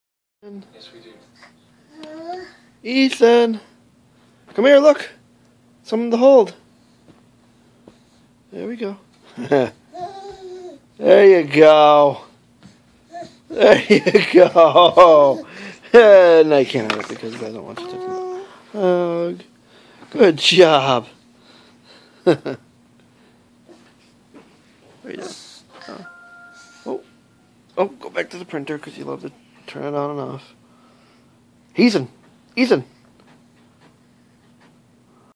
665 - Ethan Perfects his Walking
Ethan walks from one side of the room, to me on the couch. He is a pretty good walker now. Now if I can say his name right the first time, and not the second time, like at the end of the video. lol